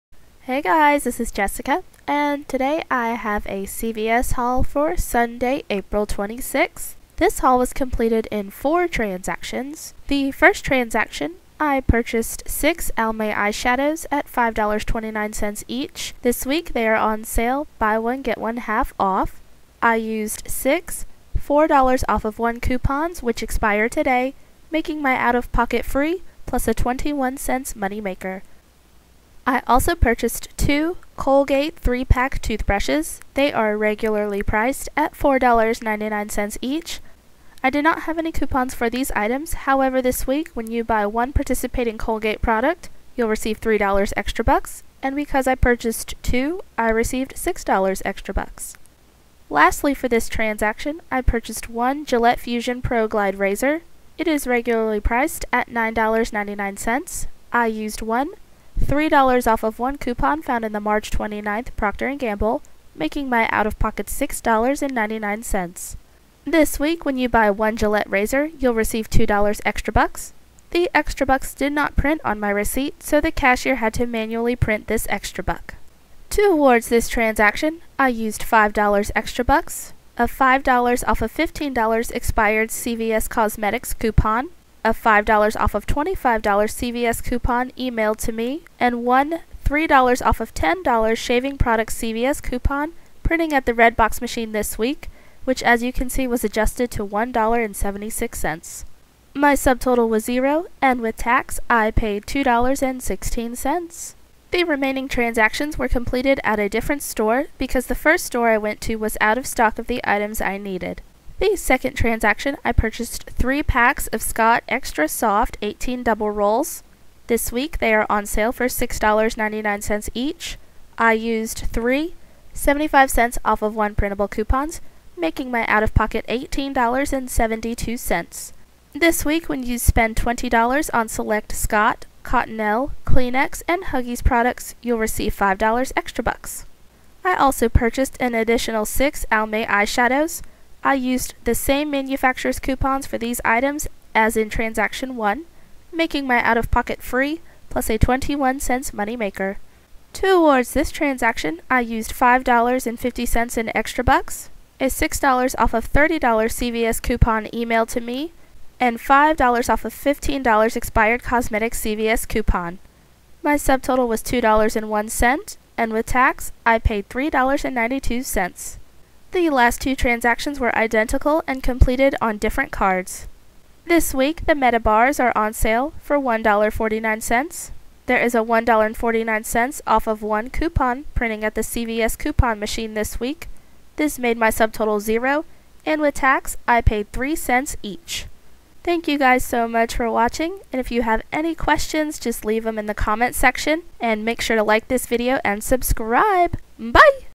CVS Haul 4/26/15
Transaction 1:
Almay $5.29 x 6 = $31.74
Used (6) $4/1 in 4/12 SS
Final = FREE + $0.21 Moneymaker!

Colgate $4.99 x 2 = $9.98
Earned $6ECB

Gillette $9.99
Used $3/1 in 3/29 PG
Final = $6.99 + earned $2ECB

Towards transaction:
$5 ECB
$5/$15 CVS cosmetics purchase
$5/$15 CVS purchase
$3/$10 CVS shaving products
Subtotal: $0, after tax: $2.16

Transactions 2
Scott Extra Soft $6.99 x 3 = $20.97
Used (3) $0.75/1 ip
Final = $18.72 + earned $5 ECB

Almay (see Transaction 1)

Towards transaction
$5.50 ECB
$6/$30 CVS purchase
$5/$15 CVS cosmetics
Subtotal: $2.01, after tax: $3.92

Transactions 3 and 4:
Meta Bars $1.49
Subtotal: $0, after tax: $0.03 each